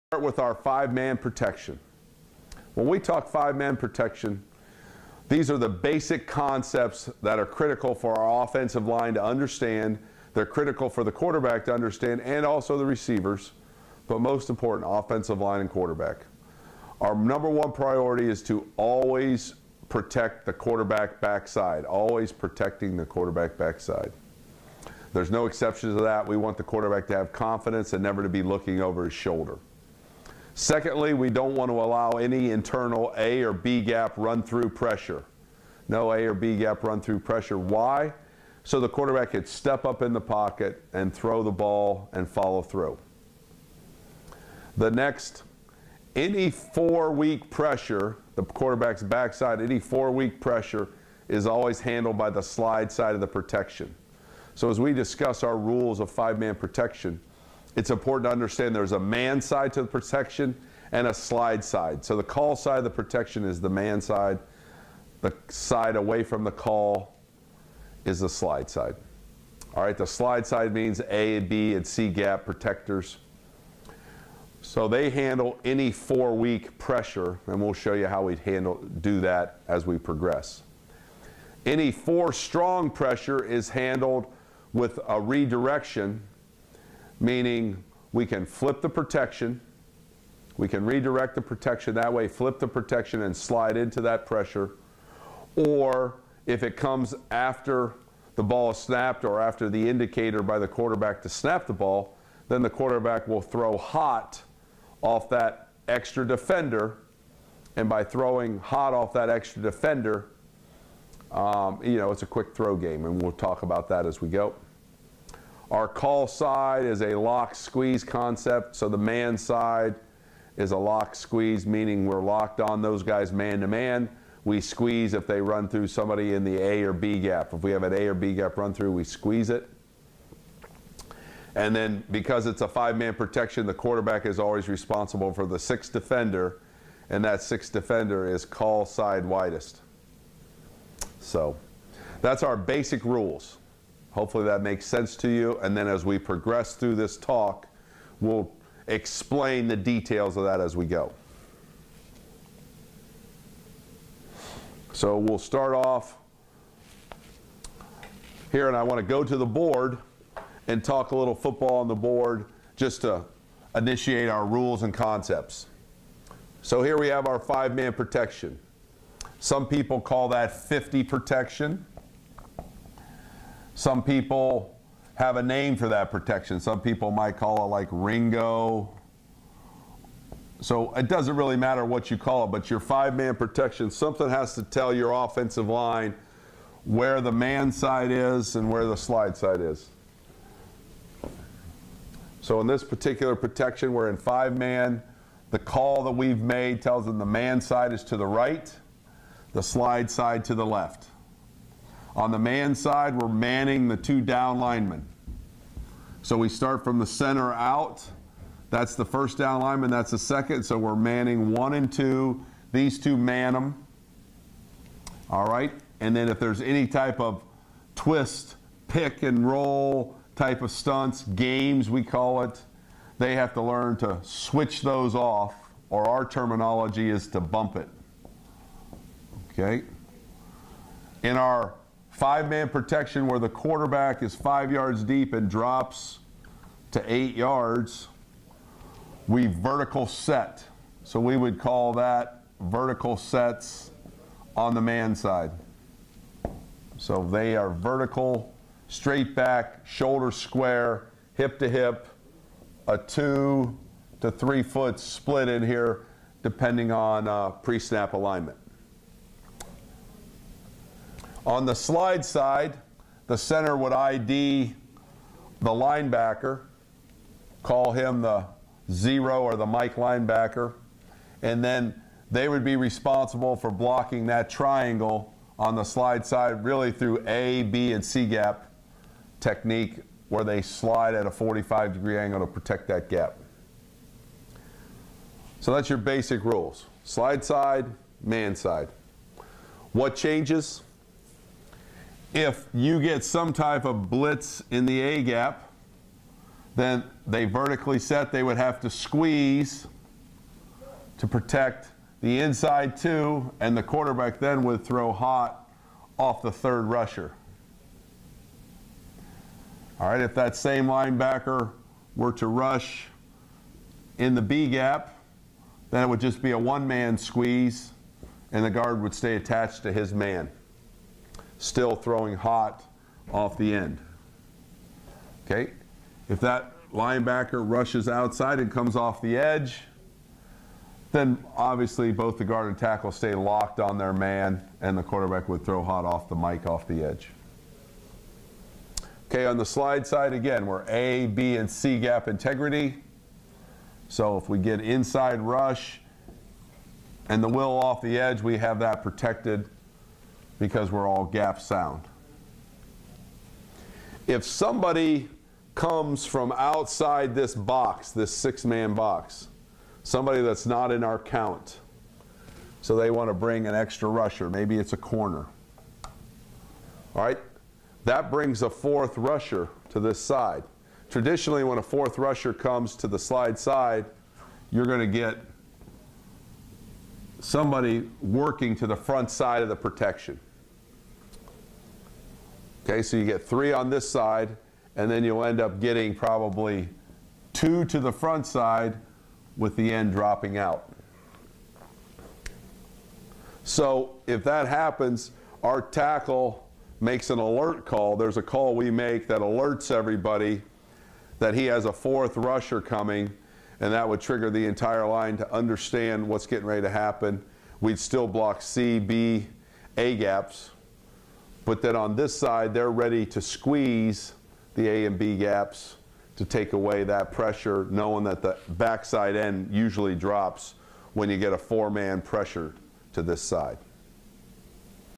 5 Man Pass Protection
5 Man Pass Protection

When we talk 5 Man Protection these are the basic concepts that are critical for our offensive line to understand they're critical for the quarterback to understand and also the receivers but most important of offensive line quarterback. Our number 1 priority is to always protect the quarterback backside always protecting the quarterback backside there's no exception to that. We want the quarterback to have confidence and never to be looking over his shoulder...

Get more videos like this one by getting a Season Pass 5 Man Pass Protection by Coach Warinner!